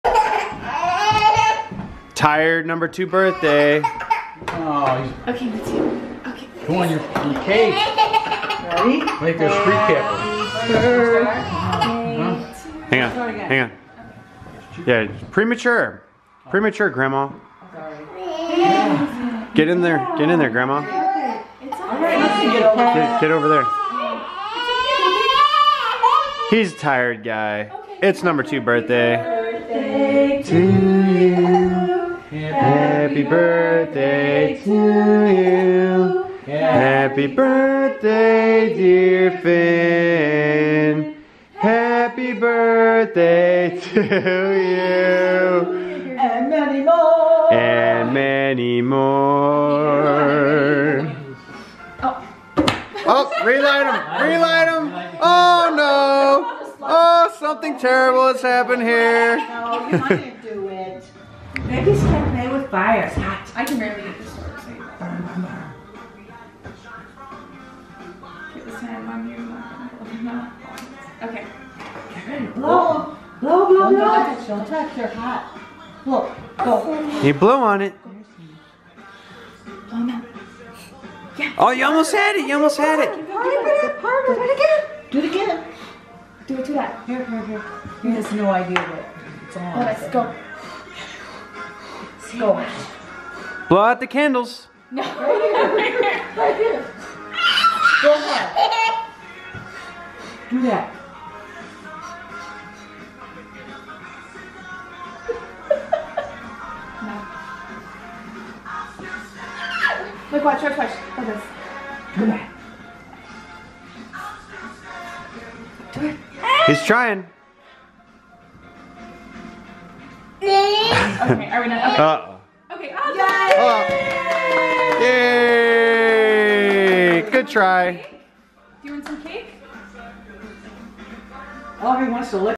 Finn #2 Birthday and candle training
We try to train a very tired Finn on how to blow out his candles at #2 birthday party. This is really just the small family pre-party. We had a really big taco-themed birthday party at Cilantro's over the weekend.

Baby Finn Age: 2